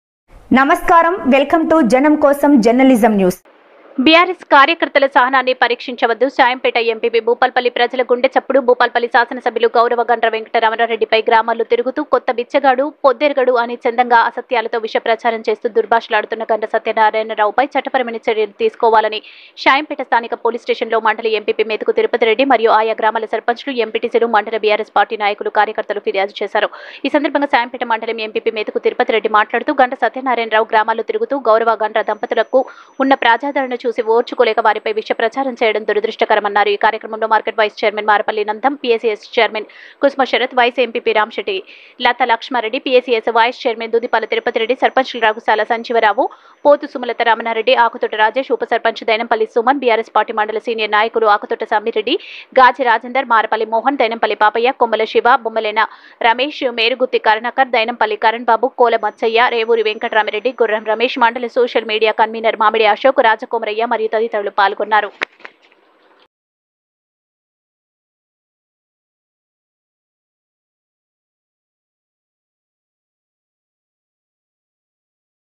భూపాలపల్లి ప్రజల గుండెచప్పుడు *భూపాలపల్లి శాసనసభ్యులు గౌరవ గండ్ర వెంకటరమణ రెడ్డి గారిపై* గ్రామాల్లో
*బిఆర్ఎస్ కార్యకర్తల సహనాన్ని పరీక్షించవద్దు. శాయంపేట ఎంపీపీ*

భూపాలపల్లి ప్రజల గుండెచప్పుడు *భూపాలపల్లి శాసనసభ్యులు గౌరవ గండ్ర వెంకటరమణ రెడ్డి గారిపై* గ్రామాల్లో తిరుగుతూ కొత్త బిచ్చగాడు పొద్దెరగడు అనే చందంగా అసత్యాలతో, విష ప్రచారం చేస్తూ దుర్భాషలాడుతున్న గండ్ర సత్యనారాయణ రావుపై చట్టపరమైన చర్యలు తీసుకోవాలని శాయంపేట స్థానిక పోలీస్ స్టేషన్లో మండల ఎంపీపీ మెతుకు తిరుపతి రెడ్డి గారు మరియు ఆయా గ్రామాల సర్పంచులు, ఎంపీటీసీలు మండల బిఆర్ఎస్ పార్టీ నాయకులు, కార్యకర్తలు ఫిర్యాదు చేయడం జరిగింది.

ఈ సందర్భంగా *శాయంపేట మండల ఎంపీపీ మెతుకు తిరుపతి రెడ్డి గారు* మాట్లాడుతూ.

గండ్ర సత్యనారాయణ రావు గ్రామాల్లో తిరుగుతూ గౌరవ గండ్ర దంపతుల గార్లకు ఉన్న ప్రజాదరణ చూసి ఓర్చుకోలేక వారిపై విష ప్రచారం చేయడం దురదృష్టకరమని అన్నారు. ప్రజల కోసం నిరంతరం కష్టిస్తూ నియోజకవర్గ అభివృద్ధిలో తమదైన పంథాలో దూసుకుపోతున్న గౌరవ భూపాలపల్లి శాసనసభ్యులు వెంకటరమణ రెడ్డి గారిపై అనుచిత వ్యాఖ్యలు చేస్తూ, అసత్యాలను ప్రచారం చేస్తూ, దుర్భాషలాడుతూ వాపు చూసి బలుపుగా విర్రవీగుతూ గ్రామాల మీదపడి విష ప్రచారానికి తెరలేపిన గండ్ర సత్యనారాయణ రావుకు  ప్రజలు ఎన్నోసార్లు బుద్ధి చెప్పారని, ఇలాంటి సిగ్గుమాలిన నాయకులను, ప్రజలు తిరస్కరించిన, సిగ్గు లేకుండా నిస్సికుగా ప్రజల్లో తిరుగుతూ కల్లబొల్లి మాటలతో ప్రజలకు దగ్గర కావాలనుకుంటున్నారు. కానీ మరో మారు ప్రజలు కర్రు కాల్చి వాత పెట్టే రోజులు దగ్గరలోనే ఉన్నాయని అన్నారు.
ఒక్క ఎకరం భూమి కలిగిన  సత్యనారాయణ రావు ఈనాడు ఇన్ని కోట్లకు ఎలా పడగలెత్తాడు, ఎన్ని అవినీతి, అక్రమాలకు పాల్పడ్డాడు ప్రజలకు తెలుసు అని అన్నారు. భూ కబ్జాలకు అక్రమాలకు పాల్పడ్డ చరిత్ర గండ్ర సత్యనారాయణది అని అన్నారు.
గౌరవ గండ్ర వెంకటరమణారెడ్డి గారు ఉన్నతమైన కుటుంబంలో జన్మించి విలువలతో బతుకుతున్నాడని వారికి ఎలాంటి అక్రమాలు, భూకబ్జాలు చేయవలసిన అవసరం లేదని అన్నారు.
ప్రజాభివృద్ధి కోసం పనిచేస్తున్న నాయకున్ని వాడు వీడు అంటూ దుర్భాషలాడడం ప్రజలు హర్షించారని అన్నారు.
గౌరవ గండ్ర దంపతులు సమాజమే దేవాలయంగా భావించి ప్రజా శ్రేయస్సే పరమావధిగా నిత్యం భూపాలపల్లి ప్రజల అభివృద్ధి కోసం పరితపించే నాయకులపై చిల్లర మాటలు మాట్లాడడం మంచిది కాదని అన్నారు.
 బిఆర్ఎస్ పార్టీ కుటుంబ సభ్యుల సహనాన్ని పరీక్షించొద్దని జిఎంఆర్ఎం ట్రస్ట్ ద్వారా అనేక సేవా కార్యక్రమాలతో ఆదరాభిమానాలు కలిగిన నాయకులపై విష ప్రచారం మానుకోవాలని హెచ్చరించారు.
 ఇలాంటివి మళ్లీ పునారవృతం అయితే తీవ్ర పరిణామాలు ఉంటాయని, బిఆర్ఎస్ కార్యకర్తల గ్రామాల నుండి తన్ని తరిమి రోజులు చూస్తావని, ఇంకా ఈ చిల్లర మాటలు ఆపకపోతో నీ  చీకటి బతుకు బయట పెడతాం బిడ్డ! అని ఘాటుగా హెచ్చరించారు.
మాట్లాడే ముందు ఒళ్ళు దగ్గర పెట్టుకోవాలని నోటికొచ్చినట్టు మాట్లాడితే చెప్పుల దండలే నీకు సన్మానం అవుతాయని అన్నారు. గౌరవం అనేది ఇచ్చిపుచ్చుకోవడంలో ఉంటుంది బిడ్డ అని హెచ్చరించారు, మా బిఆర్ఎస్ సైనికుల సహనాన్ని పరీక్షించద్దు. ఇలాంటివి ఇక పునరావృతమైతే గ్రామాలలో తిరుగనీయం ఖబర్దార్ కొడుకా! పని ఘాటుగా హెచ్చరించారు.

ఈ కార్యక్రమంలో మార్కెట్ వైస్ చైర్మన్ మారపల్లి నందం, PACS చైర్మన్ కుసుమ శరత్, వైస్ ఎంపీపీ రాంశెట్టి లతాలక్ష్మారెడ్డి, PACS వైస్ చైర్మన్ దూదిపాల తిరుపతిరెడ్డి, సర్పంచులు రఘుసాల సంజీవరావు,పోతు సుమలత రమణారెడ్డి,  ఆకుతోట రాజేష్, ఉప సర్పంచ్ దైనంపల్లి సుమన్,బిఆర్ఎస్ పార్టీ మండల సీనియర్ నాయకులు ఆకుతోట సమ్మిరెడ్డి, గాజె రాజేందర్, మారపల్లి మోహన్, దైనంపల్లి పాపయ్య, కొమ్ముల శివ,బొమ్మెలన రమేష్ మేరగుత్తి కరుణాకర్, దైనంపల్లి కరణ్ బాబు, కోల మచ్చయ్య,రేవూరి వెంకట్రామిరెడ్డి,గుర్రం రమేష్, మండల సోషల్ మీడియా కన్వీనర్ మామిడి అశోక్, రాజ కొమురయ్య మరియు తదితరులు పాల్గొన్నారు.